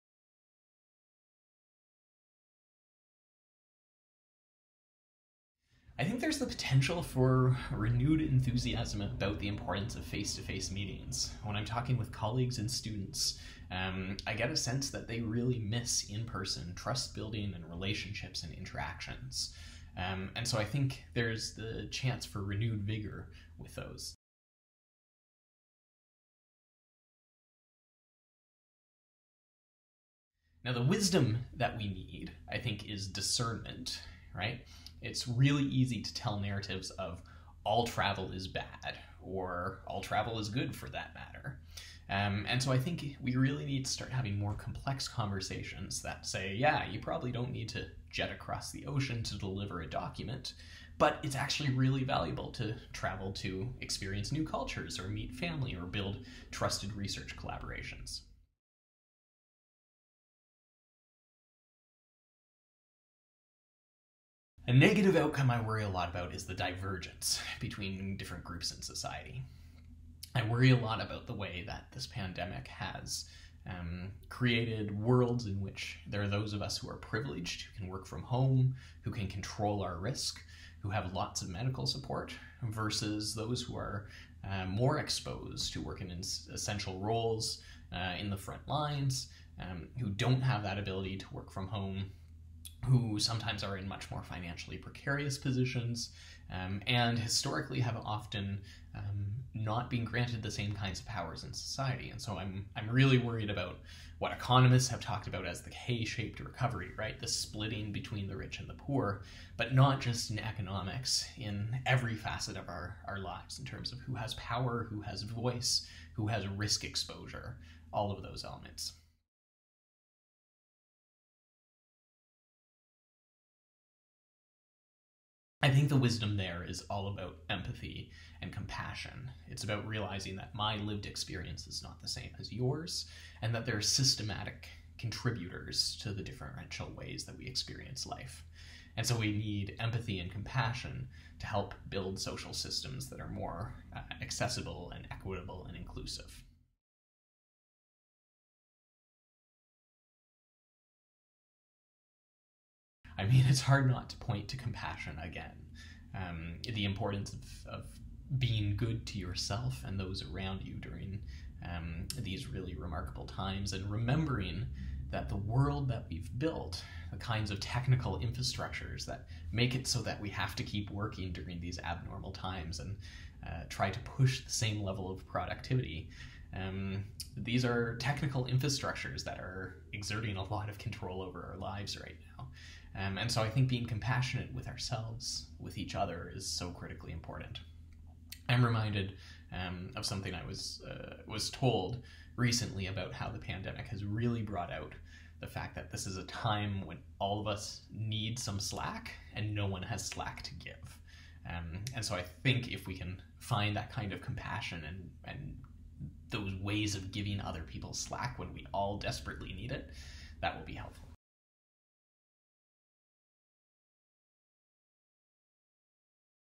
Eric Kennedy responds to five questions about the World after Covid - 7 October, 2020.
This interview is part of the larger series of conversations with world leaders in behavioral & social sciences, who documented their forecasts and advice for the post-pandemic future.

0:00 Predicted positive change in response to the pandemic.
0:25 Wisdom needed to capitalize on the positive change.
0:59 Predicted negative change in response to the pandemic.
2:00 Wisdom needed to overcome the negative.
2:28 One piece of wisdom to make it through the pandemic.

Eric Kennedy is an Assistant Professor of Disaster and Emergency Management at York University. His expertise is in the human, social, and policy dimensions of emergency management with focus on wildfire management and preparedness. He directs the Forum on Science, Policy, and Society, and leads a national project monitoring the social dimensions of COVID-19 in Canada.